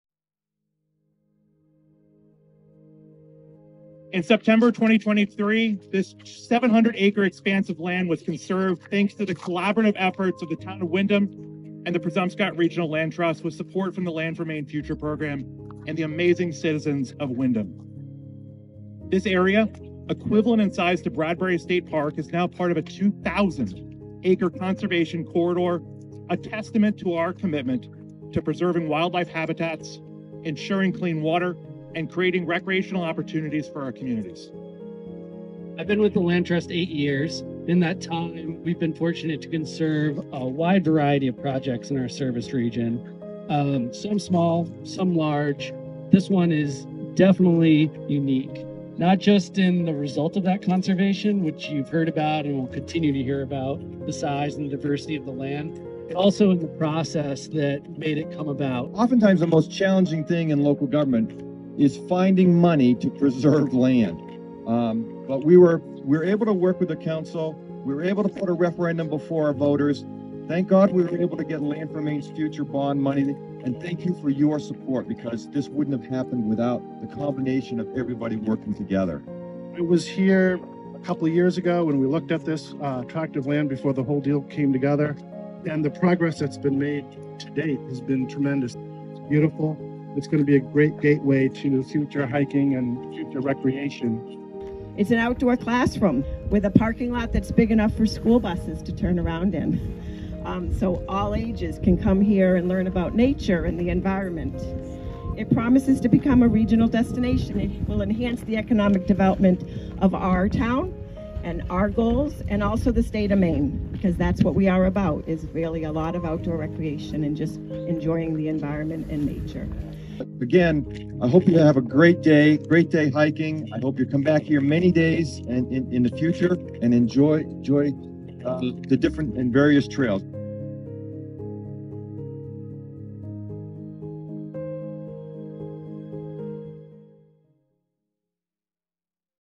East Windham Grand Opening - May 4th 2024
Coverage of the East Windham Grand Opening Event that was held May 4th 2024.